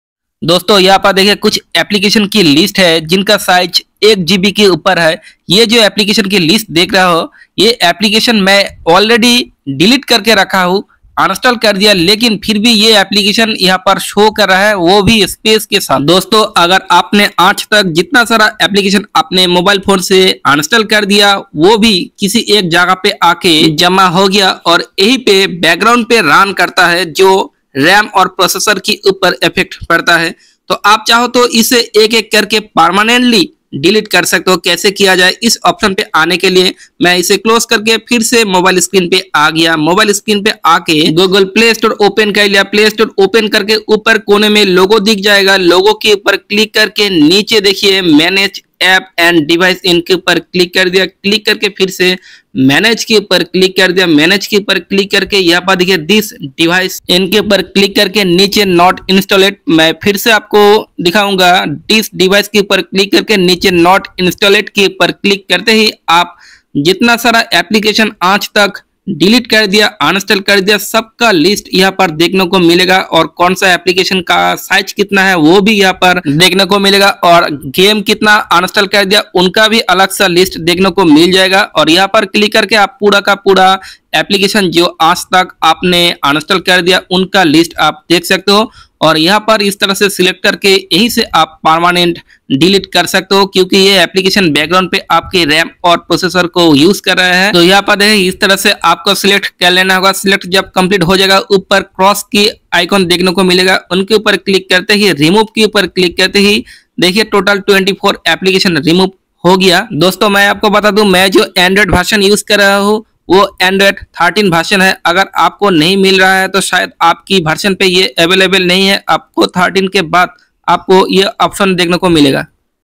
How to uninstall mobile app | How Delete Mobile app
When we uninstall or delete an app on Android mobile, in reality those apps are not deleted permanently they are stored in a specific file,
 If you want, you can go there and permanently delete your delete and uninstall apps as shown in this video.